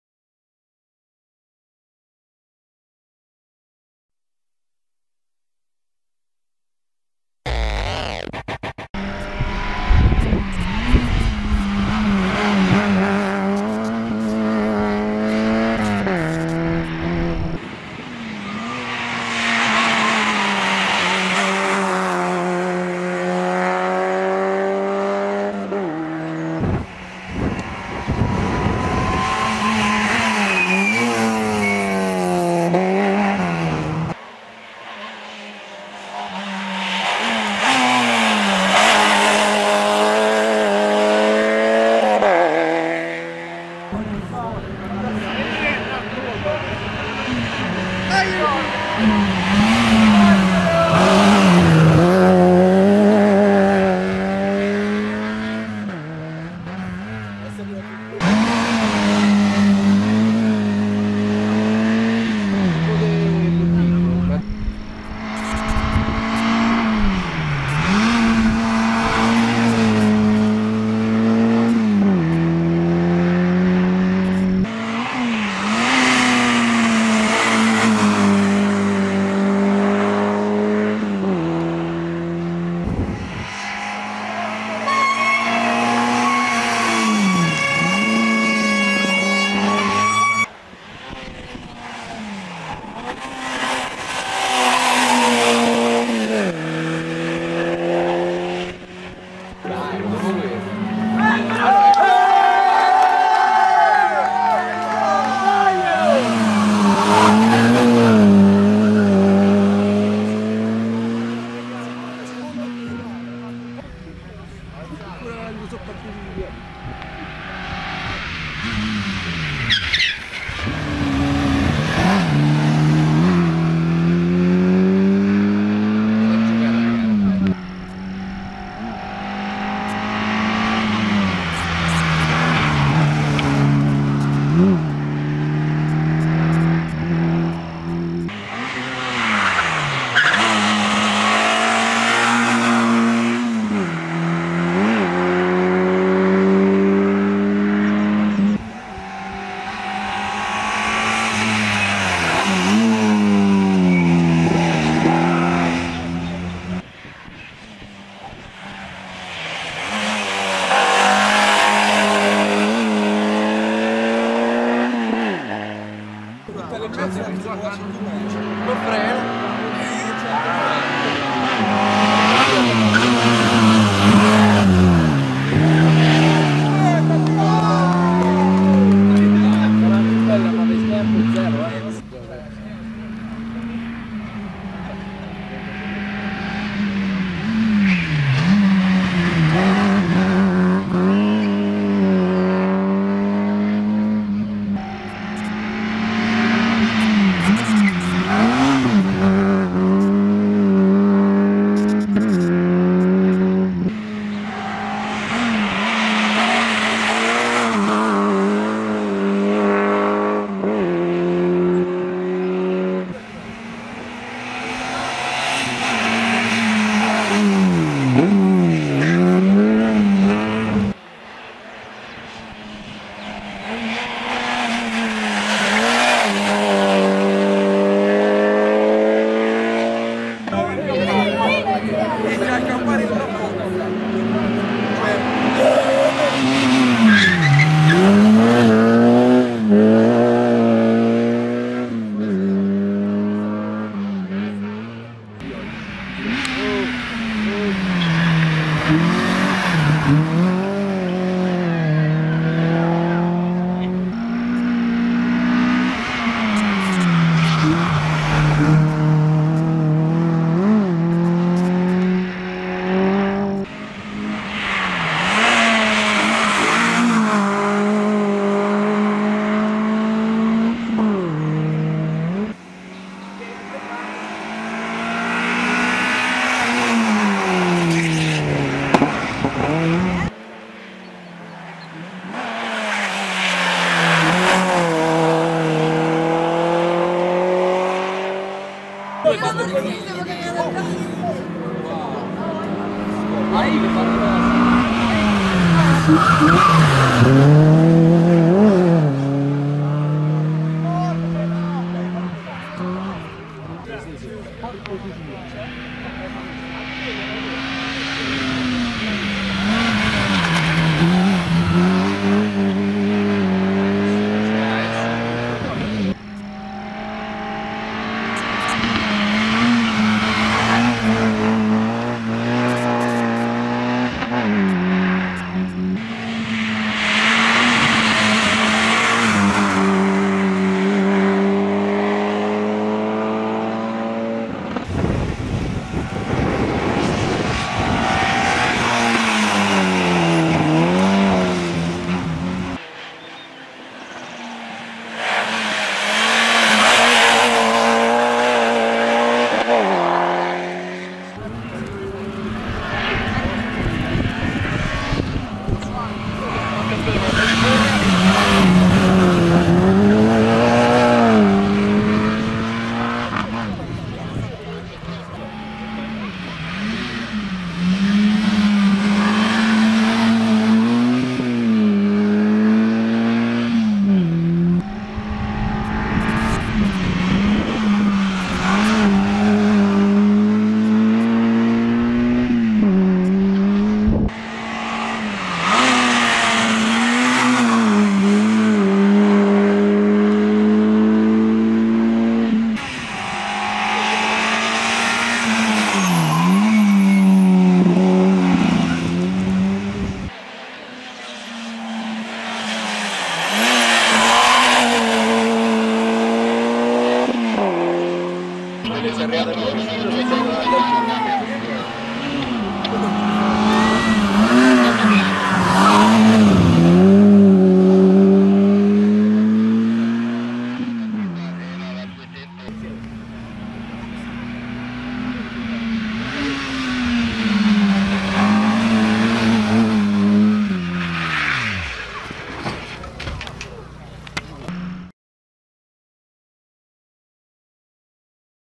COPPA CITTA' DI ORVIETO 2010 E3 1400
Nel 2010 erano incluse nel gruppo E3 ma di fatto era il gruppo N 1400. A Orvieto, era in gara un'orda di scatenati che, armati di coltello tra i denti, si diedero battaglia sul bello e impegnativo tracciato umbro appassionando il pubblico presente.